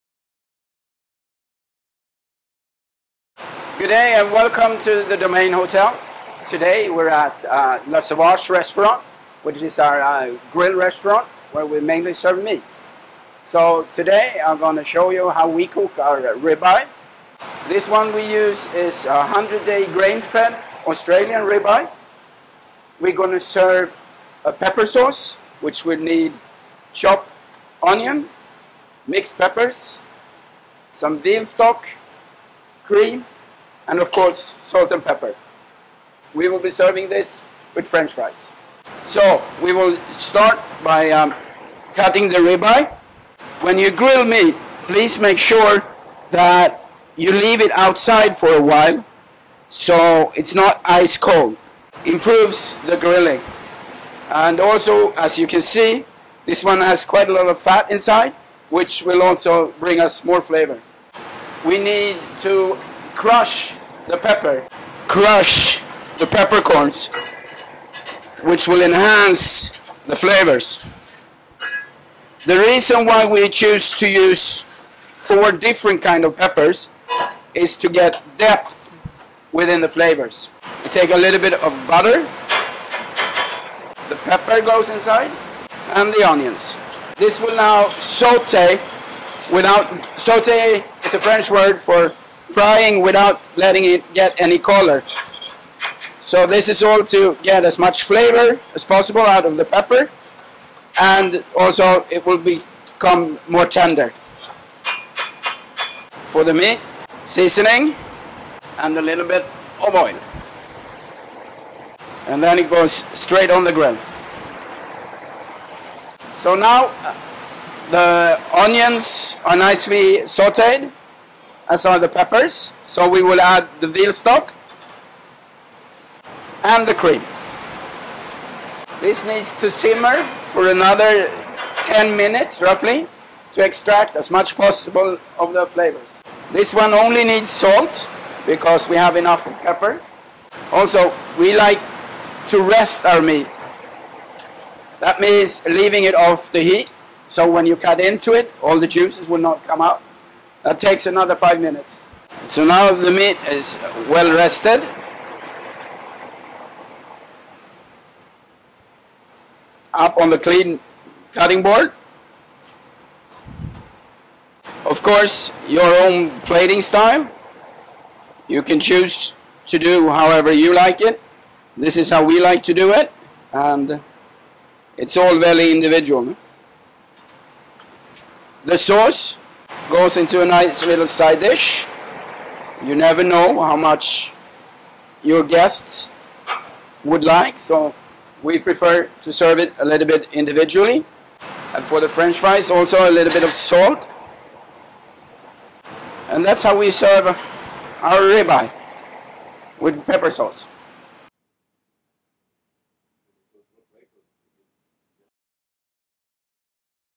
Cooking with Chef Bengt Carlsson, The Domain Bahrain Hotel
Looking for a gourmet meal without an attitude? Then Le Sauvage is your place.

Executive Chef Bengt Carlsson shares his ultimate secret Rib Eye recipe with Bahrain This Week